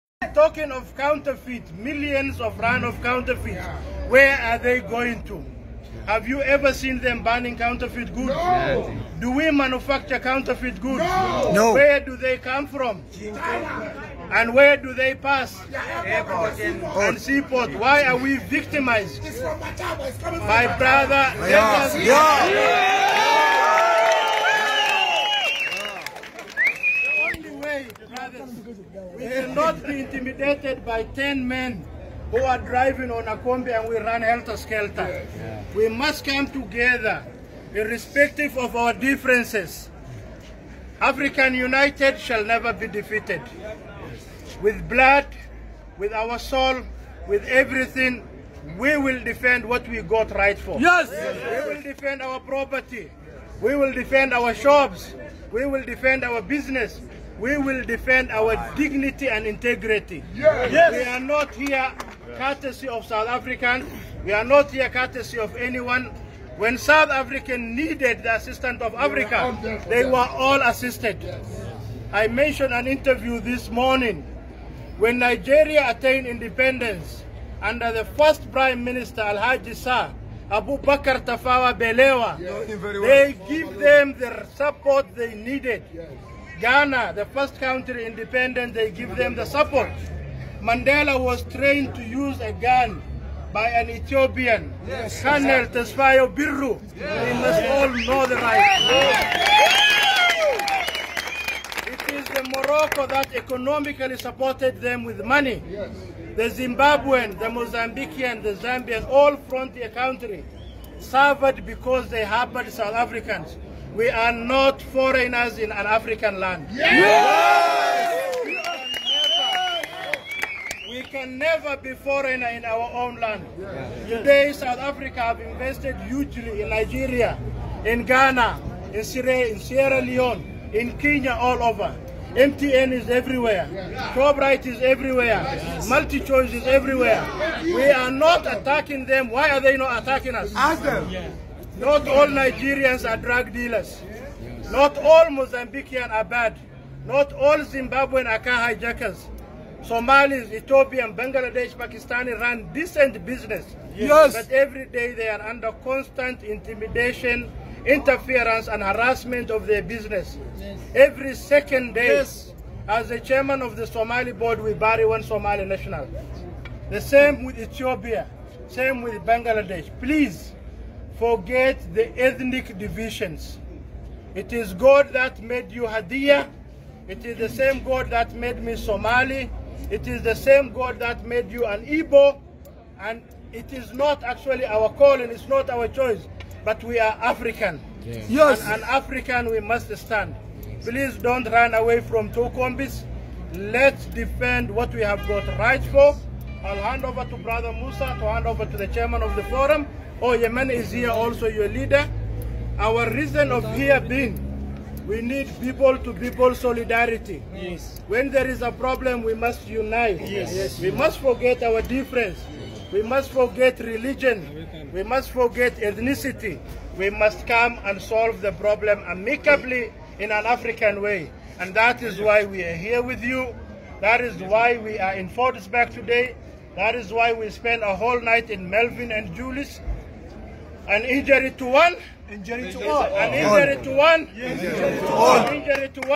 Stop Xenophobian 😤
In Africa we are not foreigners, messages from Somalian brothers.